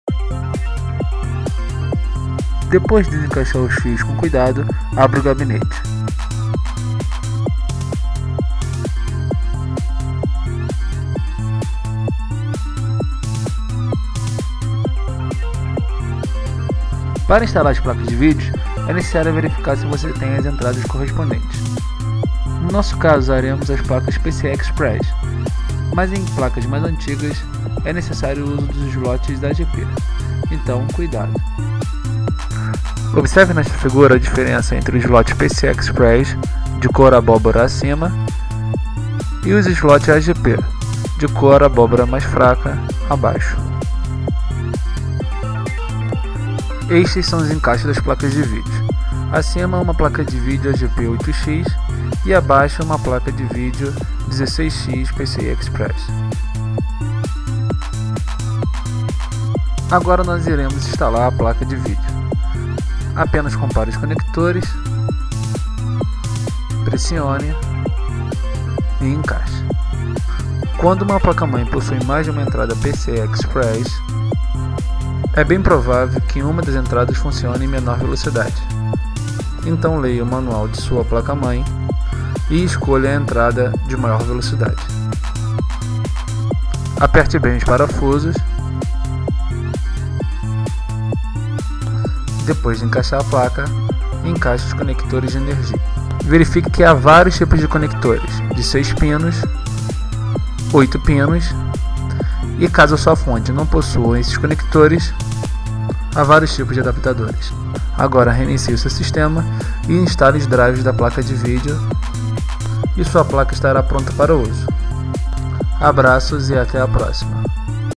Instalando Placas de Vídeo
Este vídeo visa a ensinar a usuários principiantes que nunca tiveram experiência com montagem e manutenção de micros. Neste vídeo mostraremos como se instala um placa de vídeo. Para mais informações e dicas visite o nosso site "